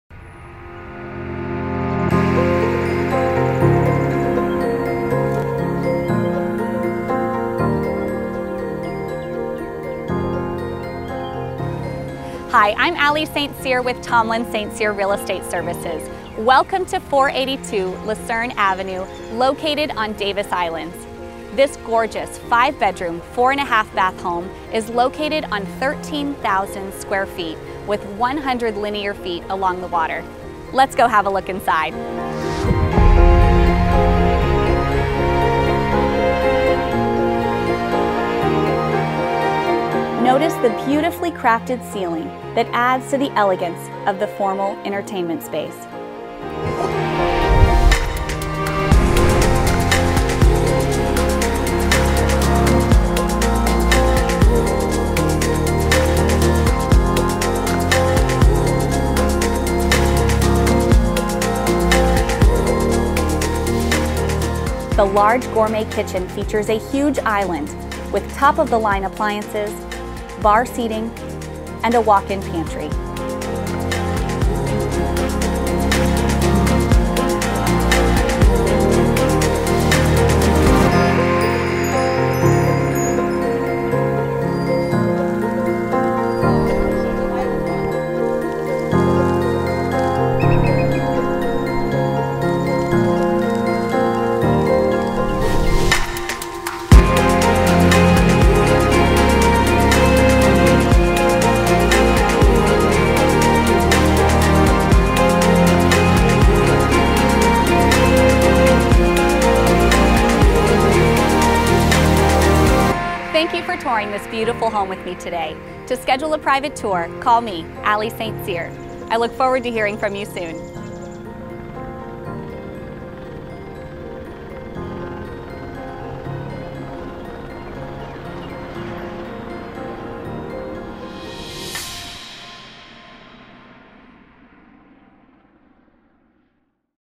Waterfront Davis Islands Home Listing Video
SOLD

A rare find on Davis Islands! This extra-large, 13,568 sqft waterfront lot claims 107 linear feet along the water, visible as soon as you walk into the grand two-story foyer. Built in the Mediterranean style, this home is 5,438 sqft, built entirely of concrete and the original roof stands in great condition. With columns and arches incorporated seamlessly into the design, this home features double crown molding, a custom iron staircase, gorgeous custom inset ceiling designs, granite countertops throughout, polished travertine floors in the kitchen and living space, absolutely no carpet and an oversized three-car garage with room for a golf cart. The gourmet kitchen features a huge center island, Thermidor stove and oven, Sharp built-in microwave, KitchenAid wood exterior, double-door refrigerator and GE wine refrigerator. Situated on the southwest side of the island, the lot is on a wide canal and has clear overhead access from bridges leading out to the bay. Approved permit for a roof to be built over the dock up to 30 feet and for a floating dock with stairs that turns into a ramp in high tide. The backyard, which extends to a spacious side yard, is visible from the large windows throughout the back of the home, which lets in tons of natural light and gorgeous sunsets. Once in the backyard, it is spacious and features a raised terrace, resort-style heated salt-water pool and spa, and a new grill with a built-in light and outdoor refrigerator.The attention to detail in this home is endless, a definite must-see!

Video Credit: ZAO Multimedia